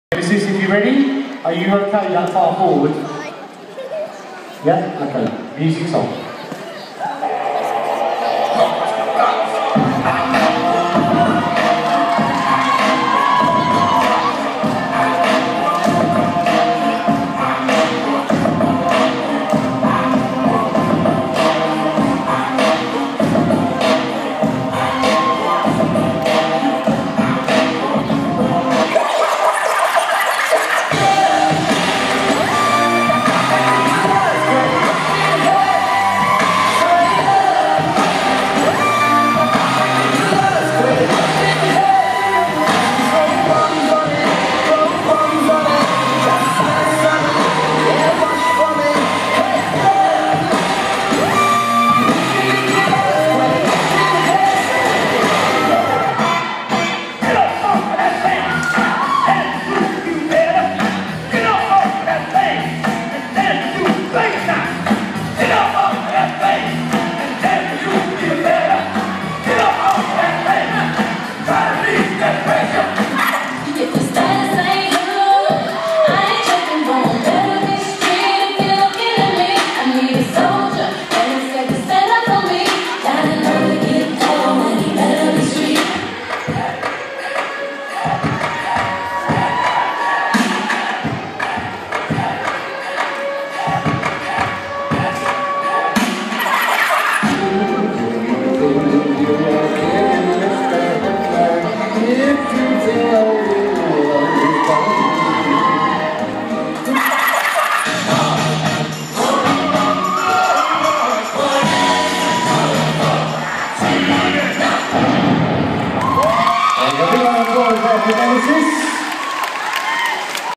SDI competition - Nemesis
Platinum dance academy Under 12's first competition